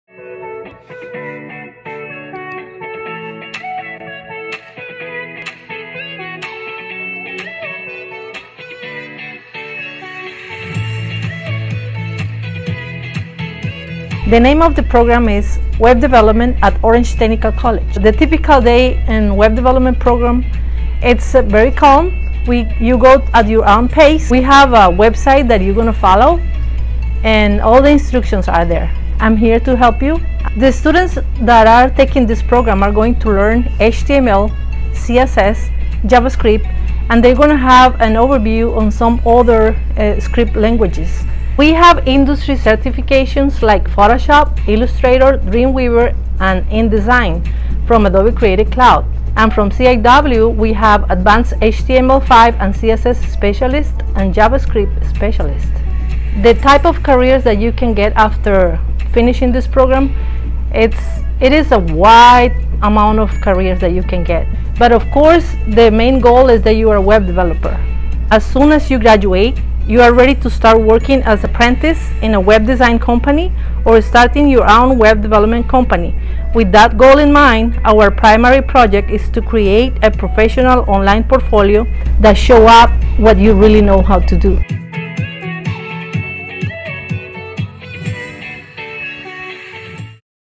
Web Development | OTC Dual Enrollment
Curious about dual enrollment at OTC? Here is a quick look inside one of our technical college classrooms and gives Orange County Public Schools high school students a preview of what to expect when signing up for a program like Web Development.